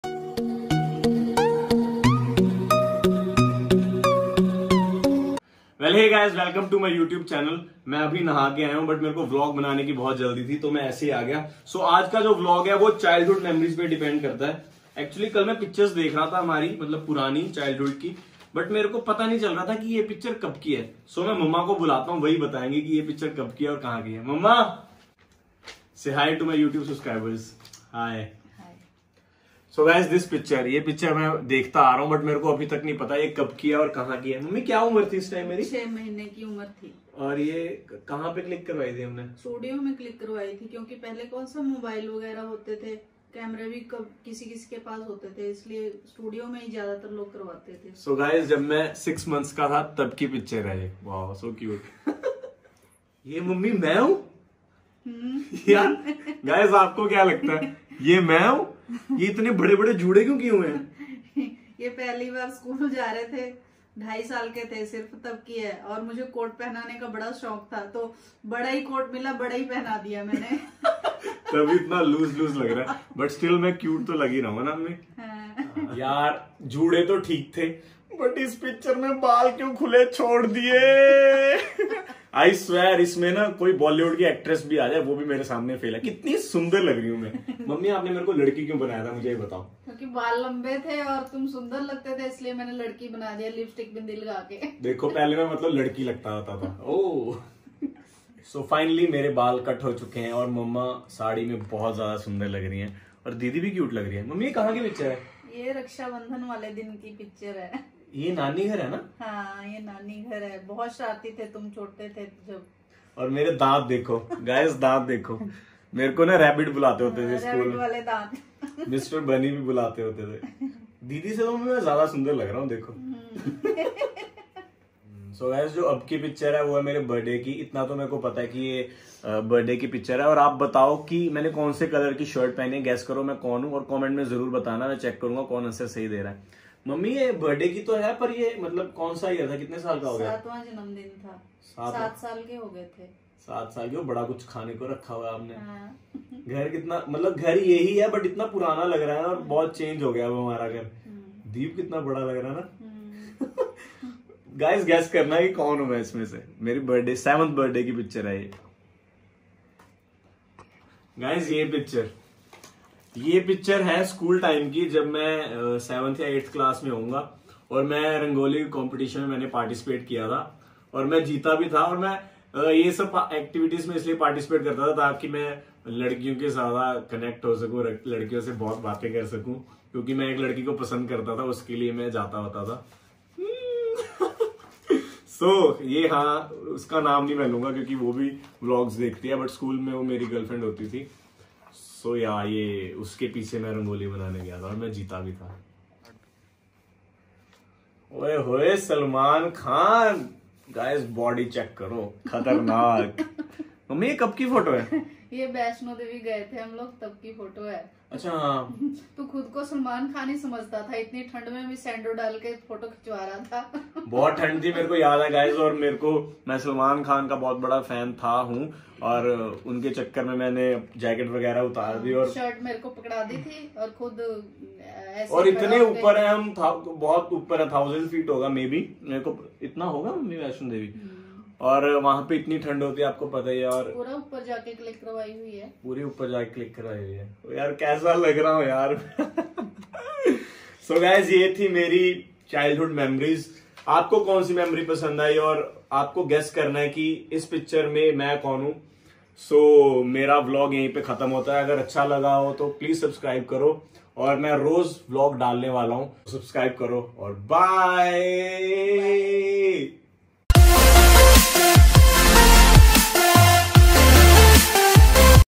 Sharing my childhood memories with mom / Abhishek kumar / Quarantine
Asking my mom about my childhood pictures